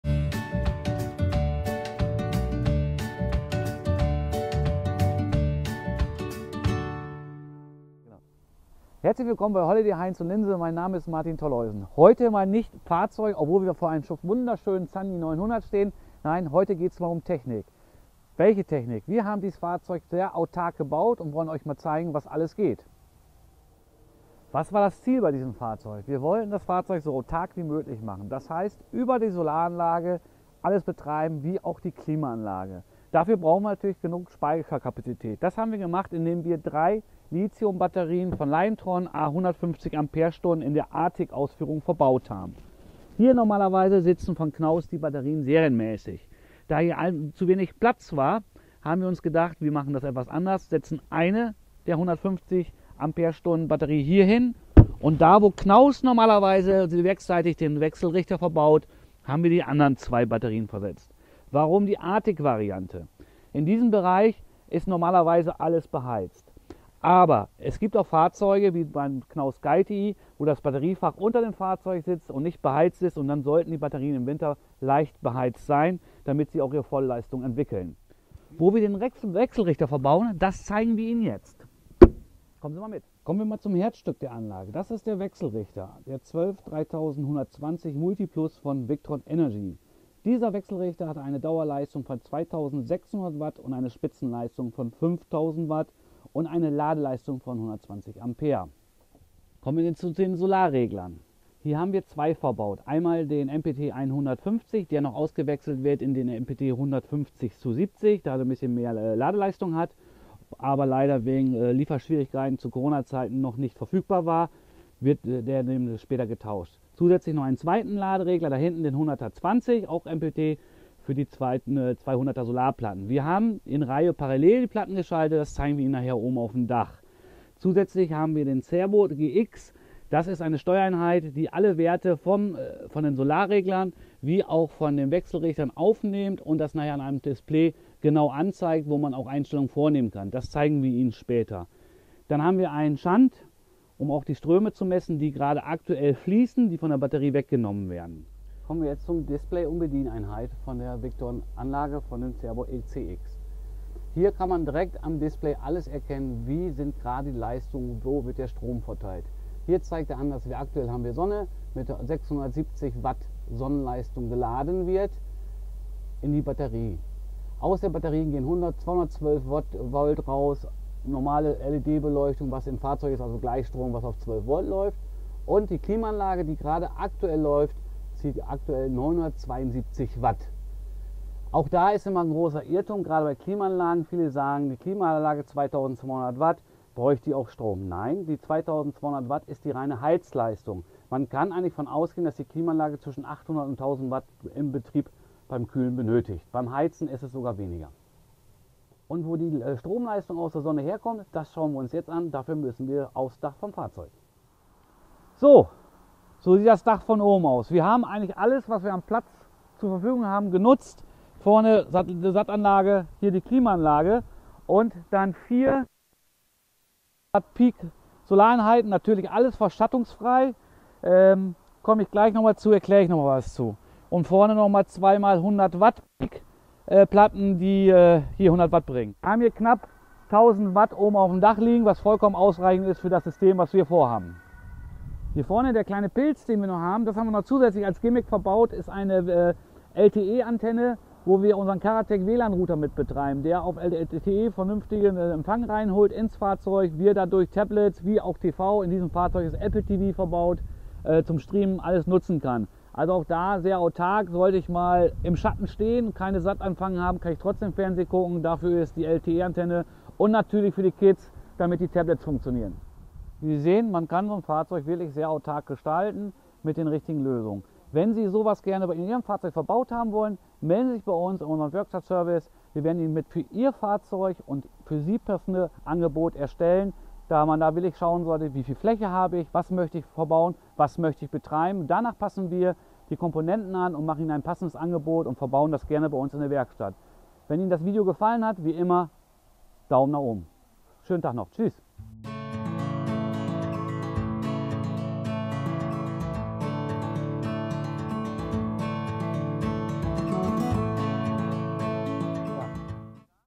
Der Autarke!!!! Sun I 900 LEG 2021 mit 1000WP Solar und 3KW Wechselrichter
In diesem Video geht es um die Technik die wir in diesem Sun I 900 LEG verbaut haben.

Es befinden Sich 4x 195WP und 2x 100WP Solar auf dem Dach des Fahrzeuges.
Weiter wurde von Victron Energy der Cerbo GX und das Cerbo Touch verbaut. Die Zentrale der Anlage!! Diese verbindet alle Solarregler und auch den Wechselrichter von Victron miteinander.
Der Wechselrichter ist ein Vicntron Energy Multi Plus 12/3000/120-16 mit 2600Watt Dauerleistung

Wenn Sie auch so ein Angebot haben möchten melden Sie sich einfach bei uns.